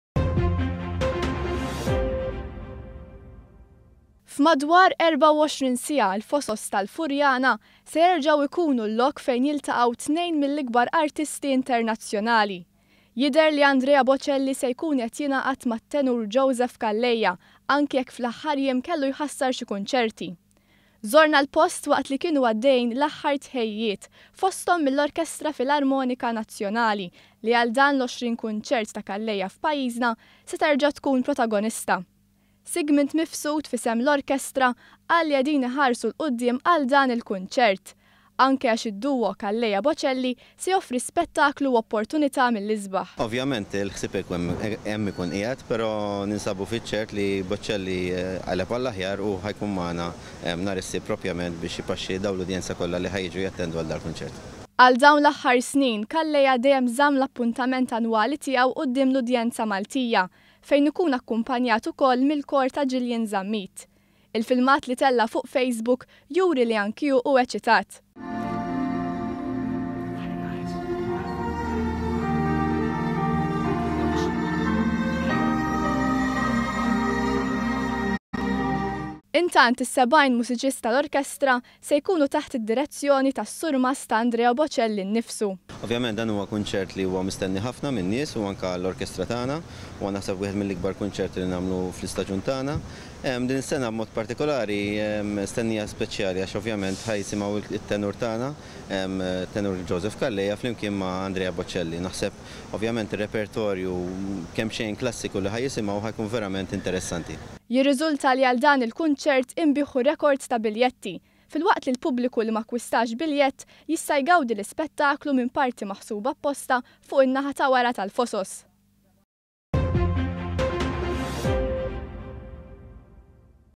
IL-FOSOS LESTI GĦAL BOCELLI U CALLEJA.
ONE.COM.MT • Għada fuq il-Fosos tal-Furjana ser isir il-kunċert tant mistenni bejn tnejn mill-ikbar aritsti internazzjonali – it-tenur Andrea Bocelli u Joseph Calleja. It-tenur malti reġa zamm l-appuntament annwali tiegħu sabiex fl-lejla sajfija jerġa jpaxxi lill-udjenza maltija